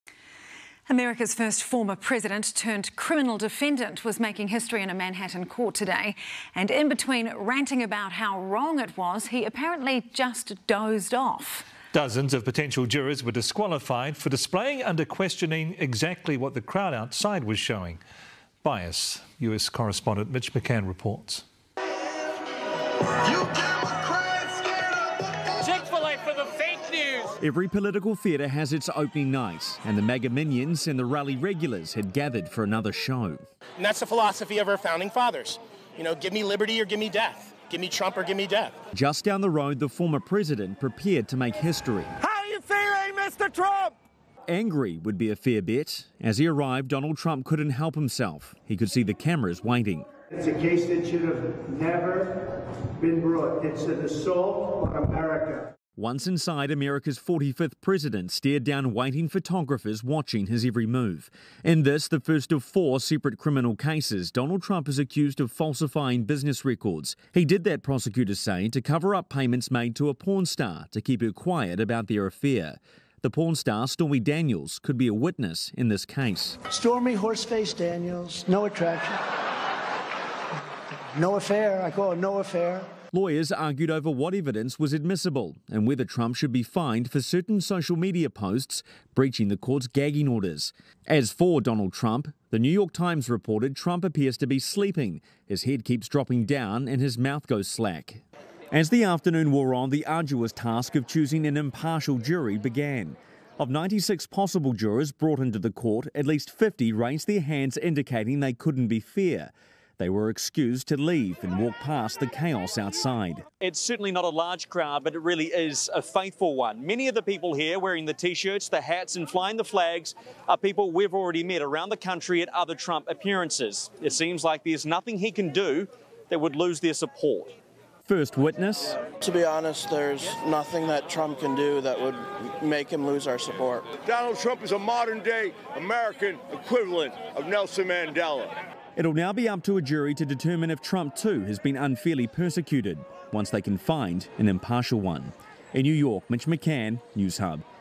'Assault on America': Reports Donald Trump dozed off during first day of criminal trial | Newshub
America's first former President turned Criminal Defendant was making history in a Manhattan court on Tuesday, And in between ranting about how wrong it was, he apparently just dozed off. US Correspondent Mitch McCann reports.